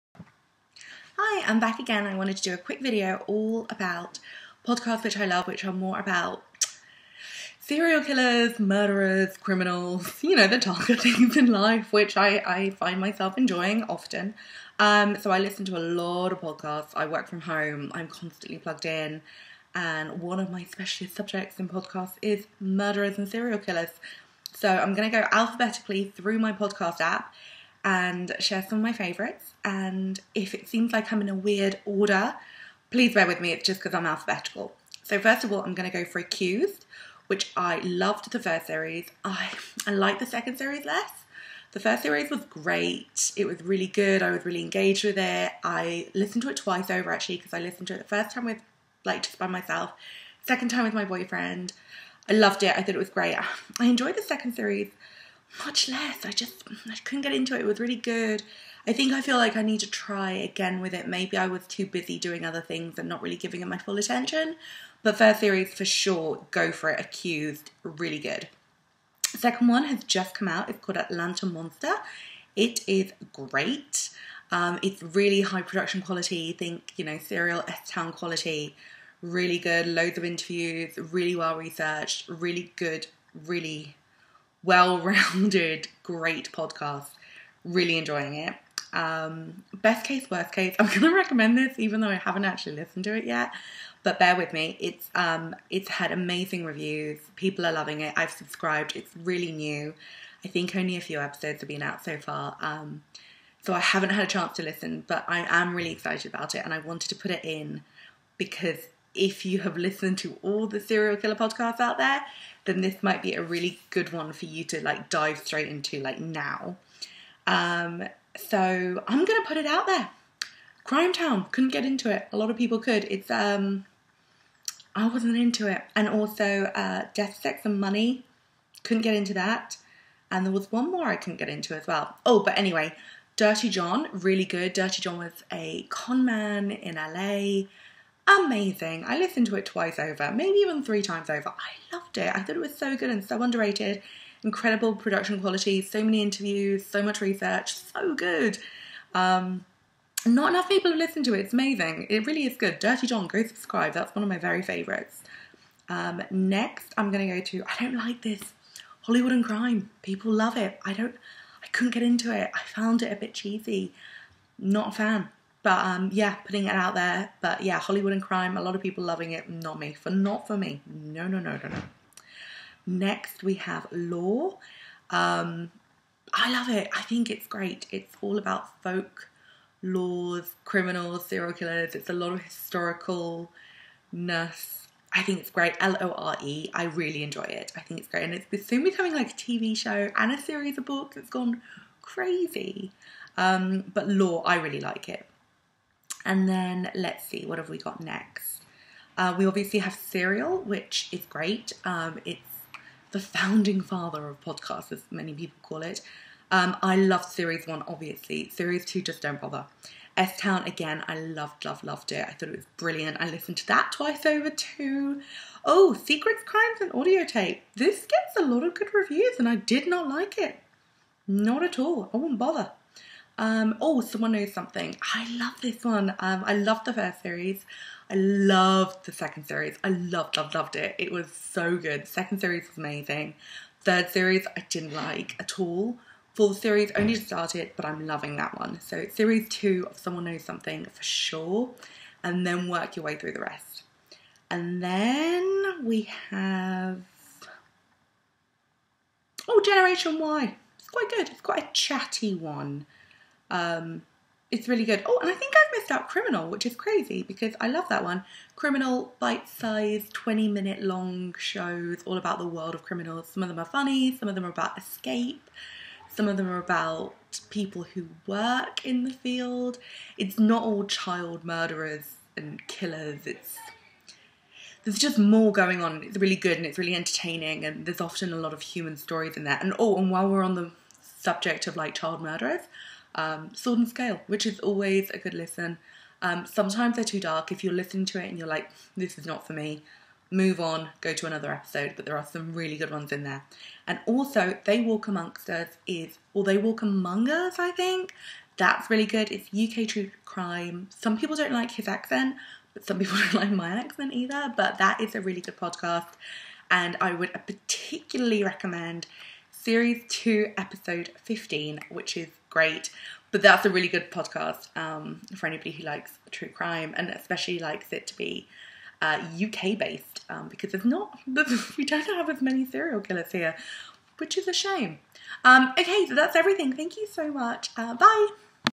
Podcasts for true crime fans! (like serial and s-town) AND which ones to avoid!
Podcasts for true crime fans! (like serial and s-town)

I have recommended loads of great podcasts here, for fans of serial, s-town and dirty john. I share which are the best episodes of accused, someone knows something, serial and so many more. Also for fans of UK true crime podcasts, 'they walk among us' is really good and i found the best episode of that podcast too. I also share which true crime podcasts you shouldn't listen to!!!